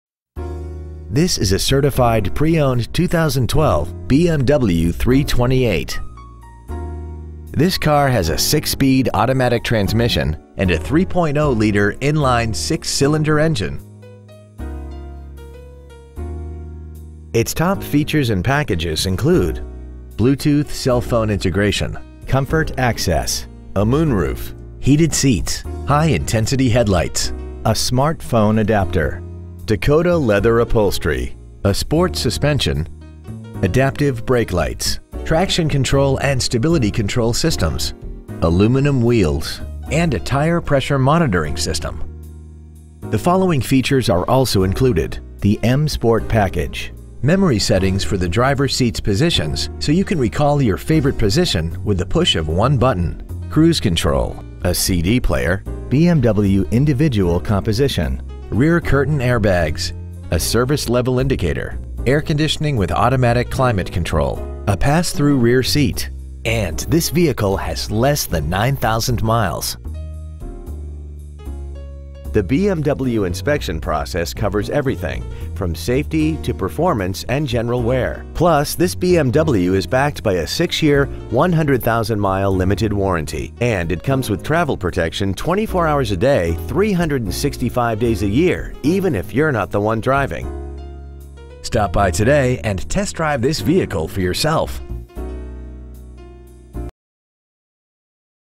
2012 BMW 328i The Woodlands TX 77384
We have been honored to serve the The Woodlands TX area , we promise that your experience at our dealership will exceed your expectations ! Year : 2012 Make : BMW Model : 328i Engine : Gas I6 3.0L/183 Trans . : Automatic Exterior : Alpine White Miles : 8,980 Interior : Black BMW MINI Houston Woodlands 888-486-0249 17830 Interstate 45 South The Woodlands , TX 77384